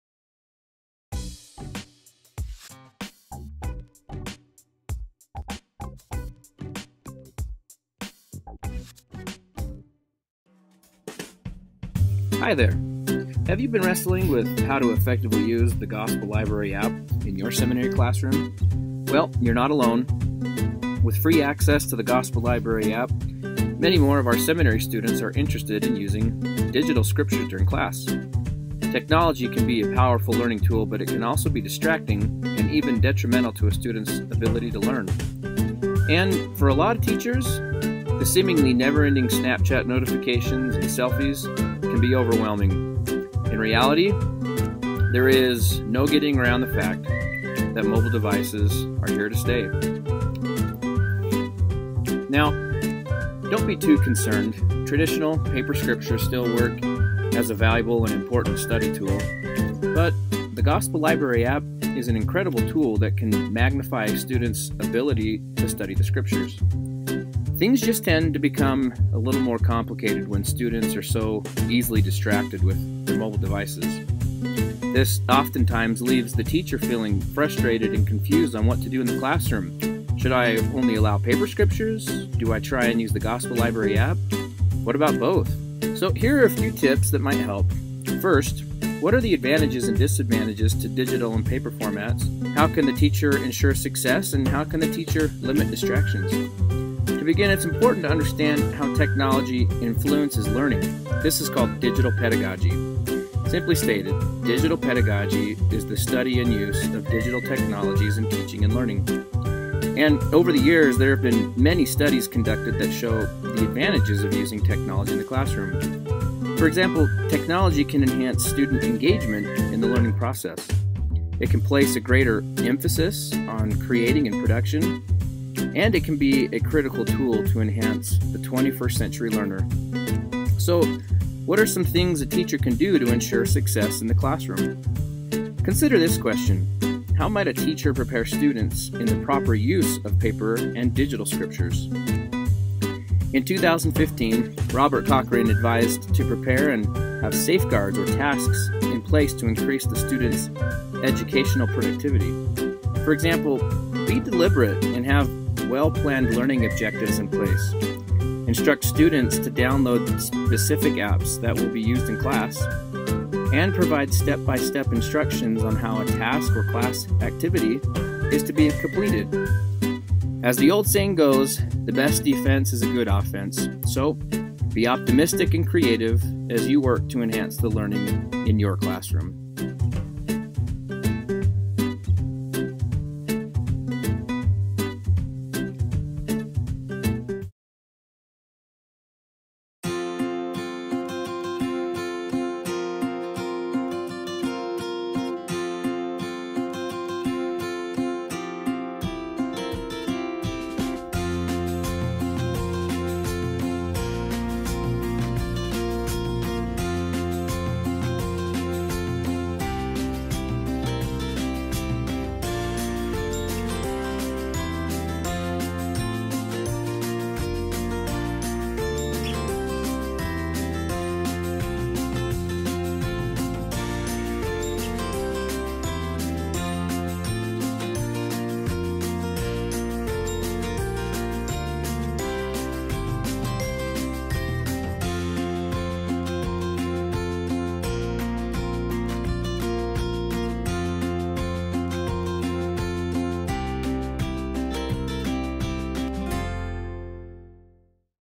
Gospel Library app
Tips for using the Gospel Library app in the seminary classroom.